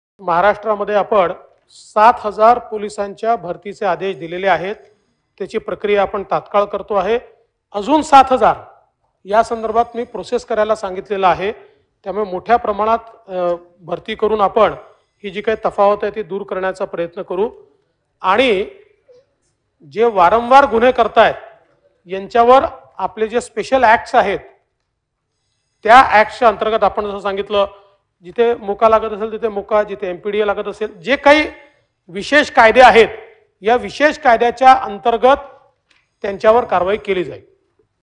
महाराष्ट्रात तात्काळ 7000 पोलिस भरतीचा आदेश | DCM Devendra Fadnavis
Instructions for recruiting 7000 police personnel are given.
Process for another 7000 will also start.
7000 पोलिस भरतीचे आदेश दिले आहेत.
आणखी 7000 पोलिस भरतीची प्रक्रिया सुरू करण्यास सांगण्यात आले आहे.
(लक्षवेधी, विधानसभा।दि. 24 ऑगस्ट 2022)

महाराष्ट्रात तात्काळ 7000 पोलीस भरतीचे आदेश
राज्यात 7000 पोलीस भरतीच्या आदेशाची तात्काळ प्रक्रिया सुरु. तसेच याव्यतिरिक्त आणखी 7000 पोलिसांची भरती प्रक्रिया सुरु करण्याचे आदेश देण्यात आले. याचसोबत जे पोलीस वारंवार गुन्हे करत असतील, त्यांच्यावर विशेष कायद्यांतर्गत कारवाई होणार असल्याची माहिती राज्याचे उपमुख्यमंत्री आणि गृहमंत्री श्री देवेंद्र फडणवीस यांनी विधानसभेमध्ये दिली.